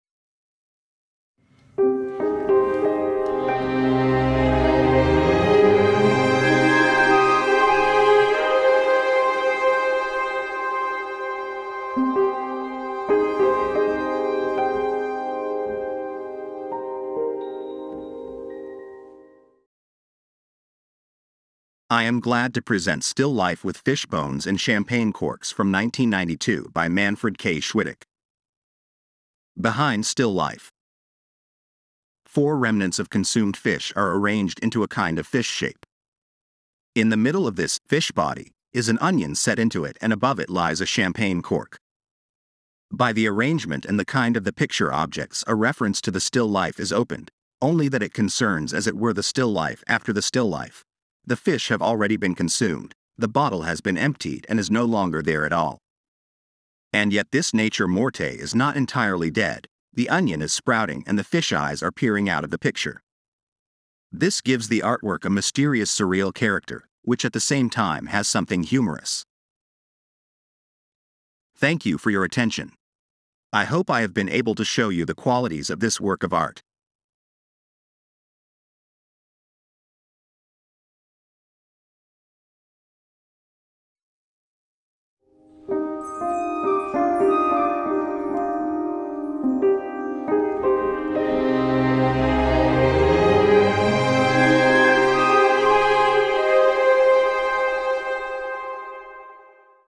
- Behind still life - / Manfred Schwitteck, Still life with fish bones and champagne corks, 1992
- Behind still life -
Manfred K. Schwitteck (*1948), Still life with fish bones and champagne corks, 1992.

Manfred K. Schwitteck (*1948), Still life with fish bones and champagne corks, 1992. watercolor over pencil on handmade paper, 31.5 x 45 cm (visible size), 47 x 61 cm (frame), signed in the center "m. k. schwitteck" and dated "26.8.[19]92", framed in passepartout under glass.

- Behind still life -

Four remnants of consumed fish are arranged into a kind of fish shape. In the middle of this 'fish body' is an onion set into it and above it lies a champagne cork.

By the arrangement and the kind of the picture objects a reference to the still life is opened, only that it concerns as it were the still life after the still life: The fish have already been consumed, the bottle has been emptied and is no longer there at all. And yet this nature morte is not entirely dead, the onion is sprouting and the fish eyes are peering out of the picture. This gives the artwork a mysterious surreal character, which at the same time has something humorous.